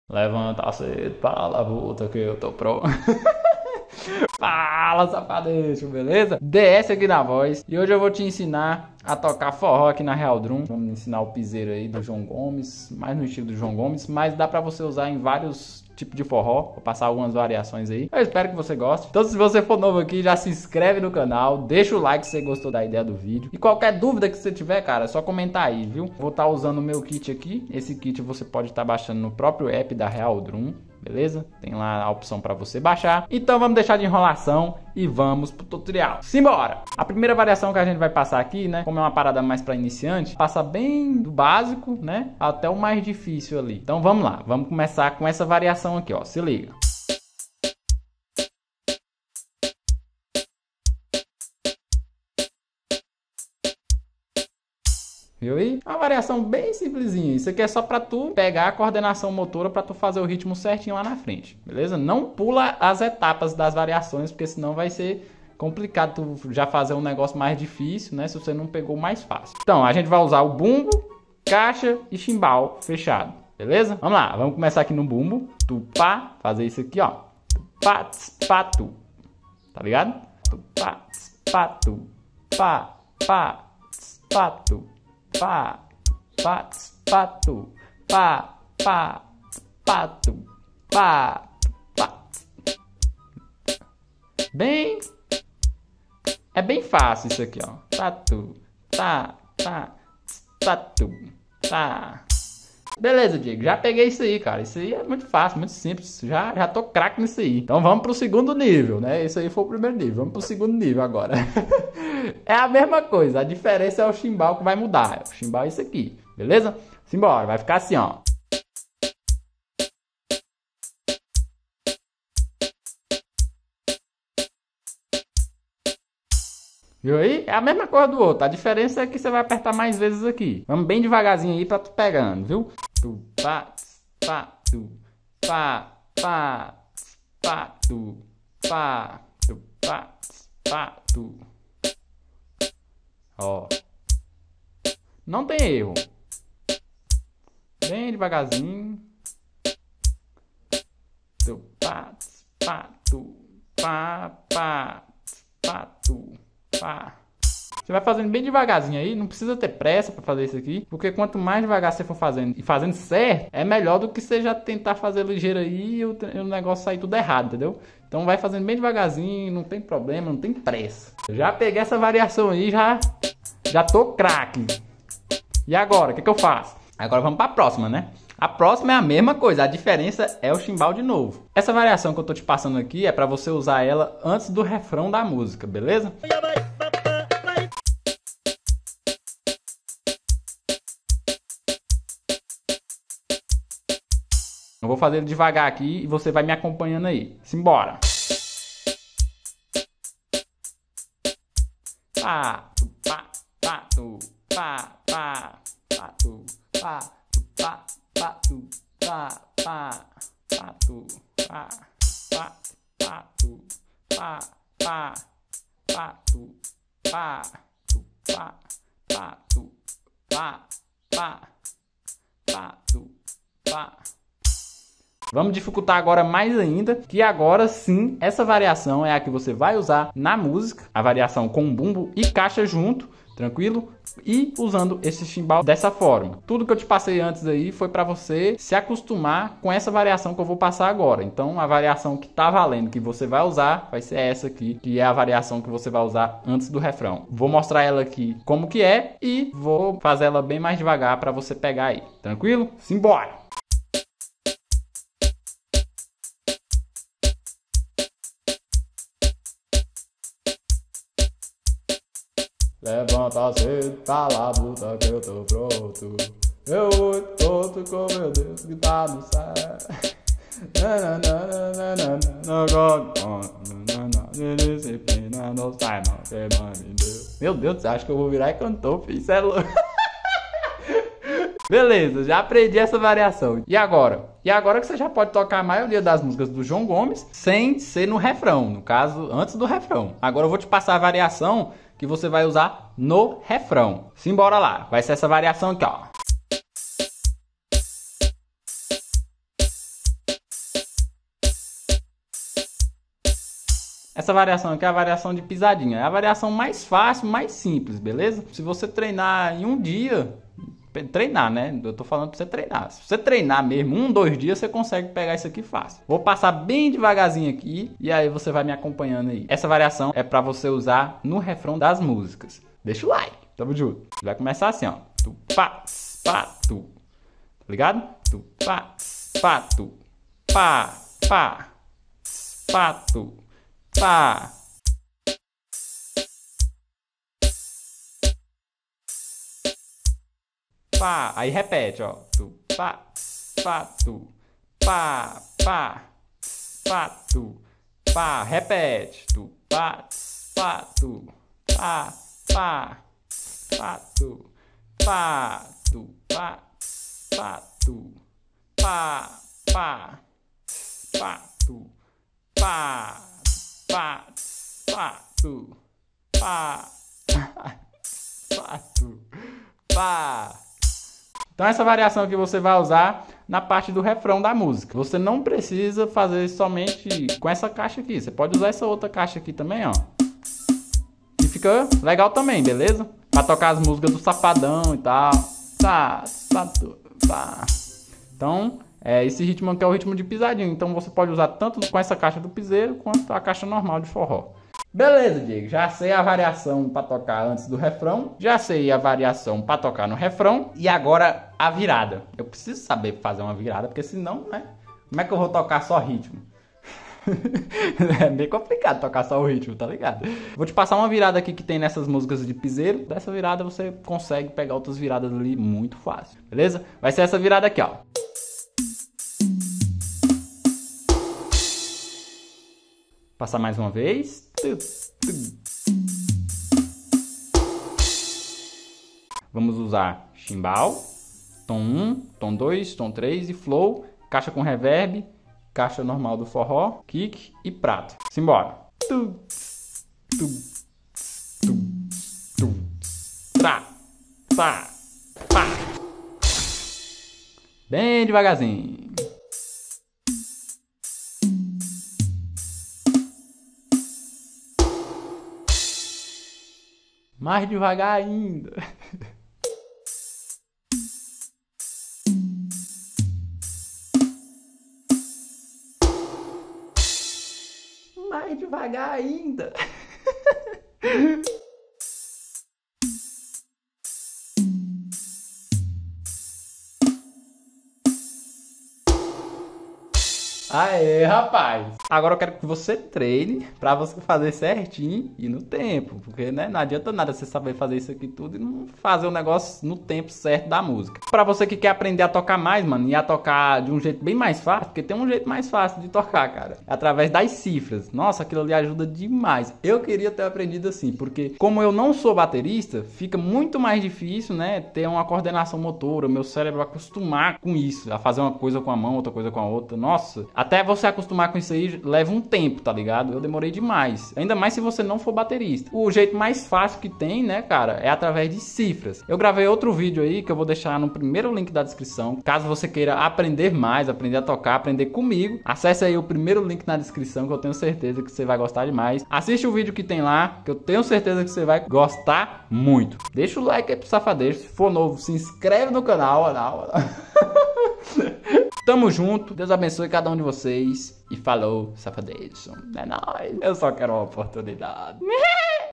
COMO TOCAR PISERO (ESTILO JOÃO GOMES) NA REAL DRUM - 2022 | (DICAS/TUTORIAL)🎶 | DIÊGO SERRACENA
🟣Conheça o "Baterista Mobile", meu curso completo para aprender a tocar bateria no celular

Aeeee somos 70k de inscritos uhuuul, pra comemorar  eu trouxe pra vocês mais 1 tutorial de Forró na Real Drum, dessa vez com o Ritmo "Pisero" estilo João Gomes , espero que goste 😆
🔴Segue no Instagrãaamm:
Minutagem do Vídeo:
00:00 - Inicio
00:28 - App Real Drum
00:47 - Exercício 1
01:57 - Exercício 2
03:06 - Exercício 3
04:12 - Variação 1 (Antes do refrão)
04:56 - Variação 2 (Usar no refrão, piseiro)
06:50 - Virada Piseiro (João Gomes)
08:11 - Como tocar de uma forma mais fácil (cifrado)
08:53 - Deixa o like safadex
09:05 - Final

▶Tutorial de como baixar o kit